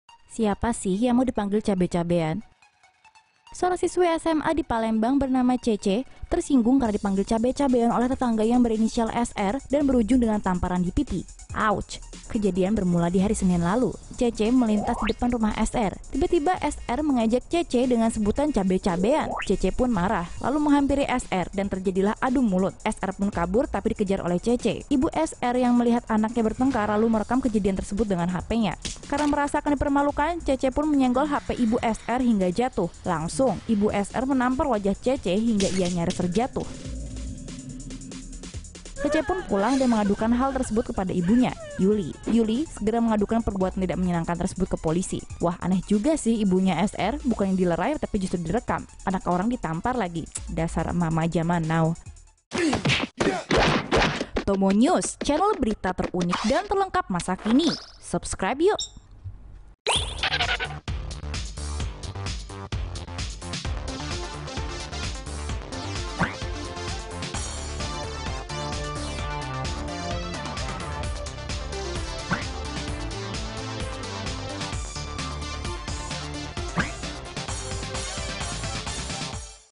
Cabe-cabean: kesal diejek cabe-cabean, berujung pada tamparan di pipi - TomoNews
PALEMBANG — Seorang siswi SMA di Palembang bernama Cece tersinggung karena dipanggil cabe-cabean oleh tetangganya yang berinisial SR dan berujung dengan tamparan di pipi.

Kejadian bermula di hari Senin lalu, Cece melintas di depan rumah SR. Tiba-tiba SR mengejek Cece dengan sebutan cabe-cabean. Cece marah lalu menghampiri SR dan terjadilah adu mulut. SR pun kabur tapi dikejar oleh Cece.

Ibu SR yang melihat anaknya bertengkar lalu merekam kejadian tersebut dengan HPnya. Karena merasa akan dipermalukan, Cece menyenggol HP ibu SR hingga jatuh. Langsung, ibu SR menampar wajah Cece hingga ia nyaris terjatuh. Cece pun pulang dan mengadukan hal tersebut kepada ibunya, Yuli. Yuli segera mengadukan perbuatan tidak menyenangkan tersebut ke polisi.